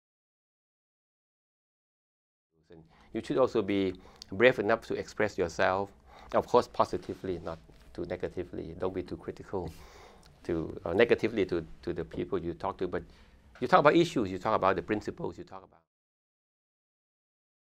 Dr. Somsak Chunharas: Be Brave Enough to Express Yourself
Dr. Somsak Chunharas spoke with the Voices in Leadership webcast program on Thursday, Sept. 7, 2017.